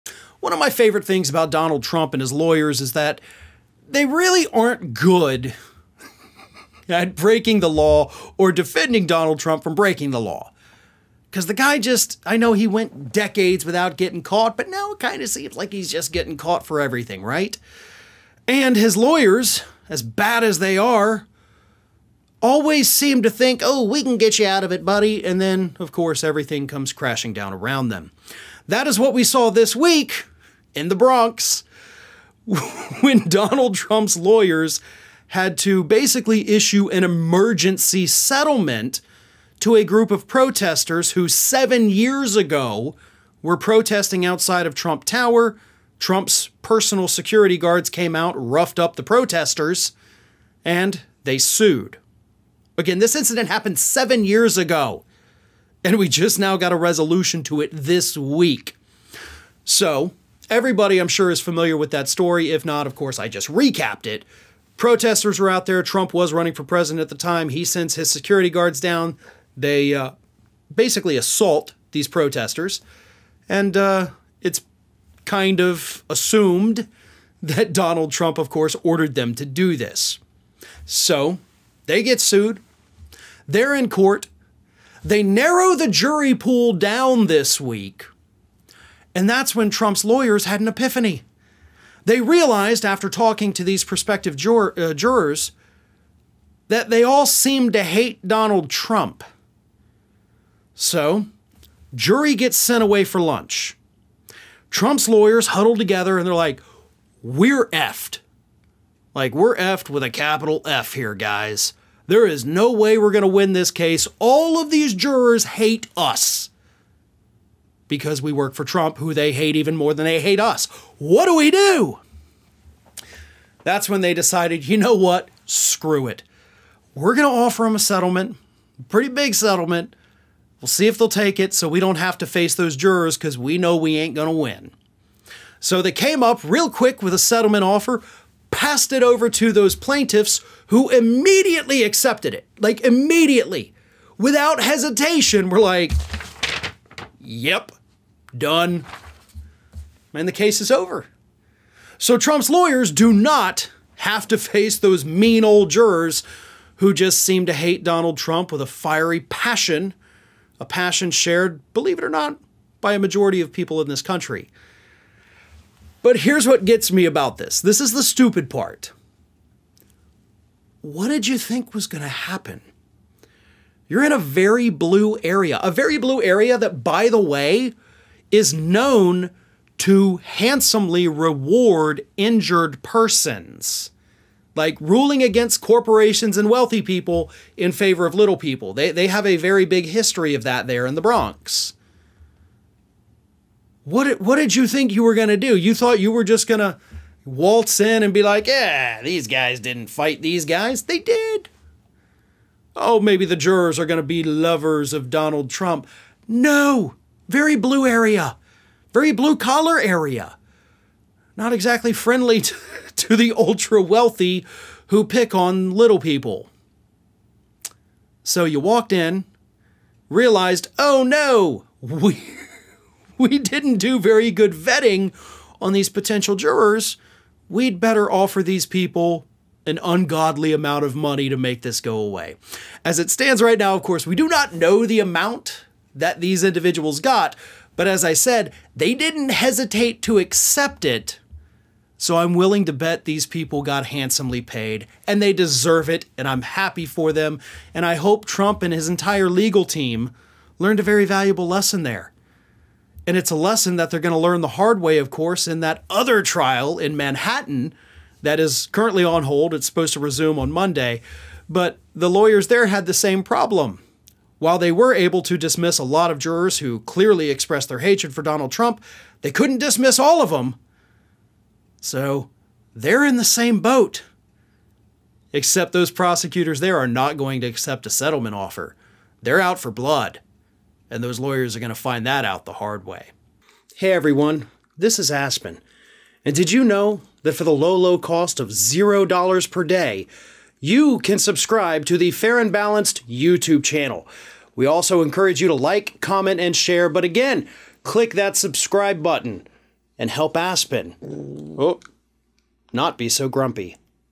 Trump Lawyers Settle Lawsuit After Realizing All The Jurors Hated Him
Lawyers for Donald Trump were forced to settle a lawsuit this week after they realized that they couldn't find any jurors who didn't hate the former President. The lawsuit involved Trump's security guards roughing up protestors many years ago, and the lawyers knew that they would have an impossible hill to climb trying to convince jurors that Trump didn't tell the guards what to do. Farron Cousins explains what happened.

One of my favorite things about Donald Trump and his lawyers is that they really aren't good at breaking the law or defending Donald Trump from breaking the law. Cuz the guy just, I know he went decades without getting caught, but now it kind of seems like he's just getting caught for everything, right? And his lawyers as bad as they are, always seem to think, Oh, we can get you out of it buddy. And then of course everything comes crashing down around them. That is what we saw this week in the Bronx when Donald Trump's lawyers had to basically issue an emergency settlement to a group of protestors who seven years ago were protesting outside of Trump. Tower Trump's personal security guards came out, roughed up the protestors, and they sued again. This incident happened seven years ago and we just now got a resolution to it this week.

So everybody, I'm sure is familiar with that story. If not, of course, I just recapped it. Protestors were out there. Trump was running for president at the time. He sends his security guards down. They, uh, basically assault these protestors and, uh, it's kind of assumed that Donald Trump, of course, ordered them to do this. So they get sued, they're in court, they narrow the jury pool down this week, and that's when Trump's lawyers had an epiphany. They realized after talking to these prospective juror, uh, jurors, that they all seemed to hate Donald Trump. So jury gets sent away for lunch. Trump's lawyers huddled together and they're like, We're effed, Like we're effed with a capital F here guys. There is no way we're gonna win this case. All of these jurors hate us because we work for Trump, who they hate even more than they hate us.

What do we do? That's when they decided, you know what? Screw it. We're gonna offer 'em a settlement, Pretty big settlement. We'll see if they'll take it so we don't have to face those jurors cuz we know we ain't gonna win. So they came up real quick with a settlement offer, passed it over to those plaintiffs who immediately accepted it. Like immediately without hesitation. We're like, Yep, done, man. The case is over. So Trump's lawyers do not have to face those mean old jurors who just seem to hate Donald Trump with a fiery passion, a passion shared, believe it or not, by a majority of people in this country. But here's what gets me about this. This is the stupid part. What did you think was gonna happen? You're in a very blue area, a very blue area that by the way, is known

To handsomely reward injured persons like ruling against corporations and wealthy people in favor of little people. They, they have a very big history of that there in the Bronx. What did, what did you think you were gonna do? You thought you were just gonna waltz in and be like, Yeah, these guys didn't fight these guys. They did. Oh, maybe the jurors are gonna be lovers of Donald Trump. No, very blue area, very blue collar area, Not exactly friendly to the ultra wealthy who pick on little people. So you walked in realized, Oh no, we. We didn't do very good vetting on these potential jurors. We'd better offer these people an ungodly amount of money to make this go away. As it stands right now. Of course, we do not know the amount that these individuals got, but as I said, they didn't hesitate to accept it.

So I'm willing to bet these people got handsomely paid and they deserve it and I'm happy for them. And I hope Trump and his entire legal team learned a very valuable lesson there. And it's a lesson that they're gonna learn the hard way. Of course, in that other trial in Manhattan that is currently on hold, it's supposed to resume on Monday, but the lawyers there had the same problem. While they were able to dismiss a lot of jurors who clearly expressed their hatred for Donald Trump, they couldn't dismiss all of 'em. So they're in the same boat, except those prosecutors there are not going to accept a settlement offer. They're out for blood and those lawyers are gonna find that out the hard.